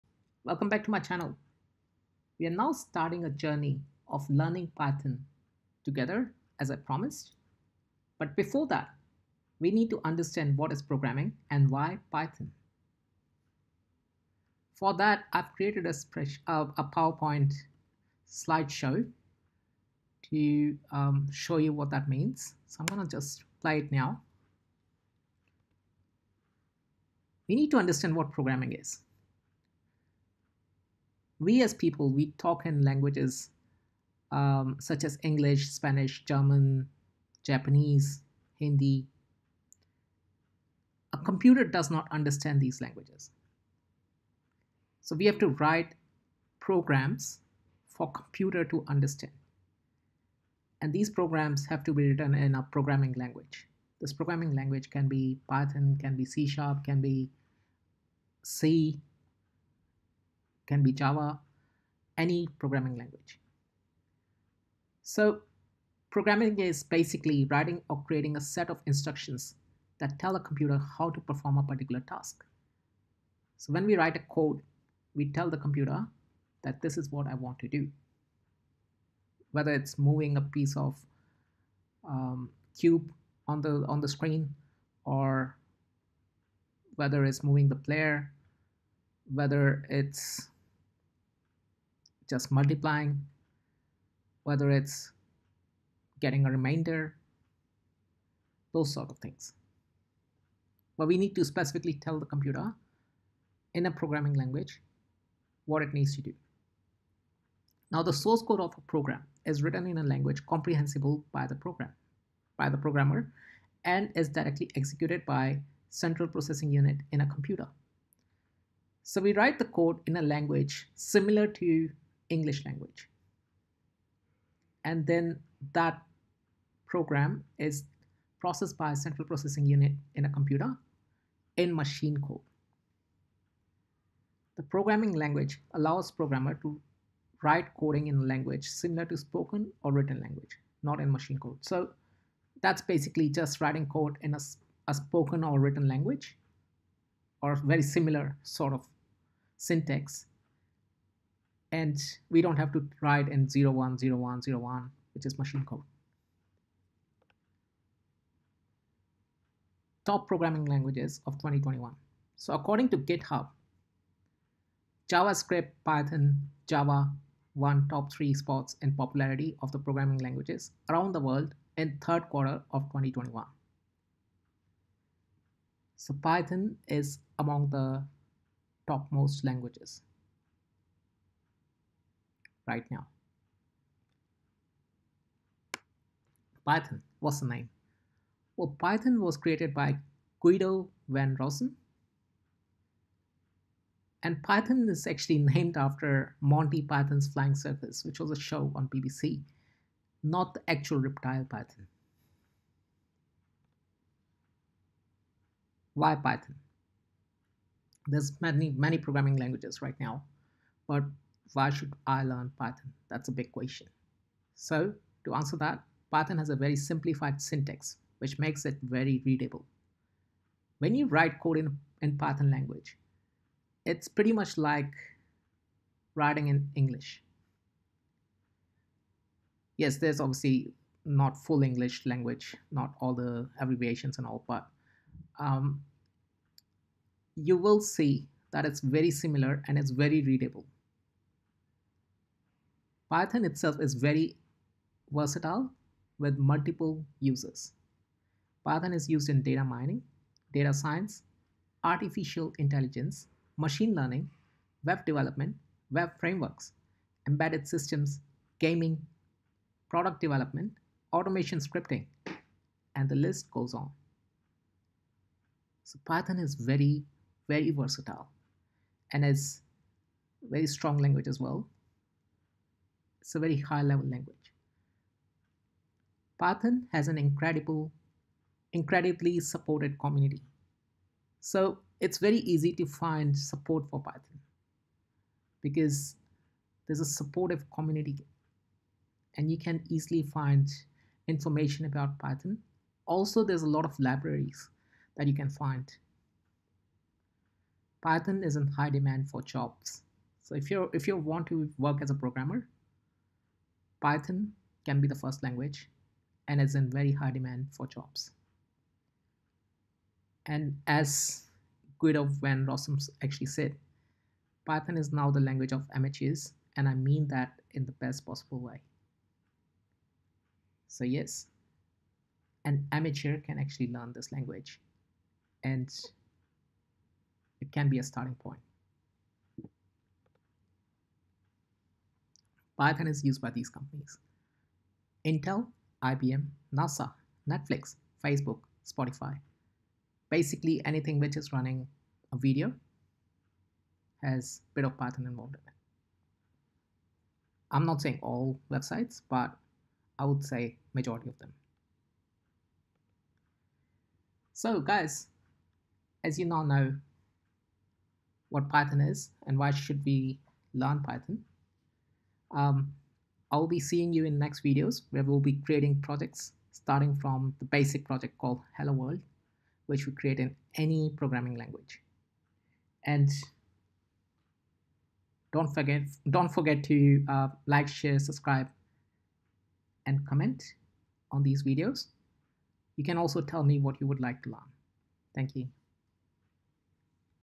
Why should I learn Python?
This is a video about what is programming and why should we learn Python. This video gives you an overview of how strong Python is and what purposes it can be used for.